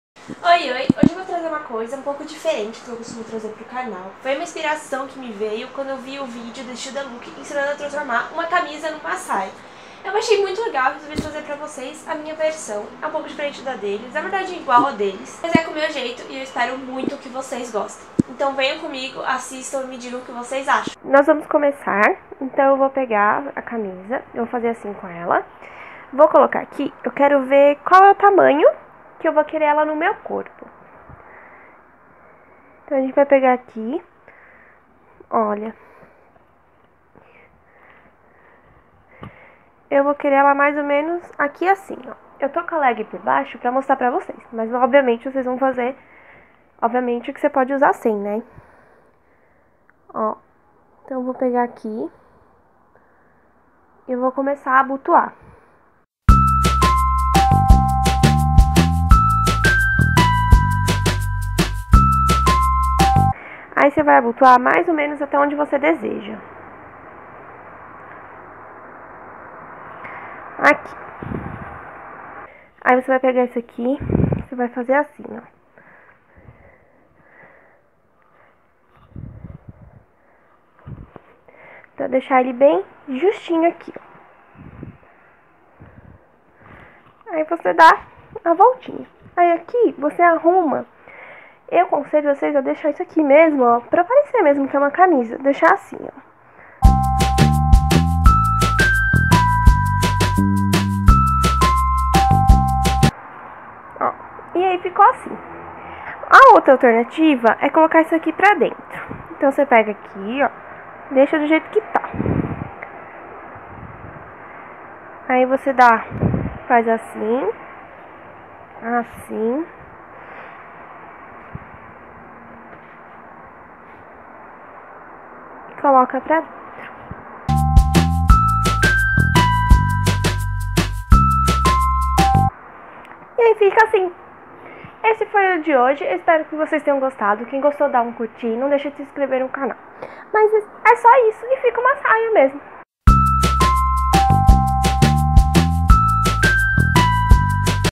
2 formas de transformar sua camisa em saia | Vestindo Ideias
Duas formas de transformar sua camisa em saia sem costurar, apenas usando da técnica de dobrar.

Snap: vestindoideias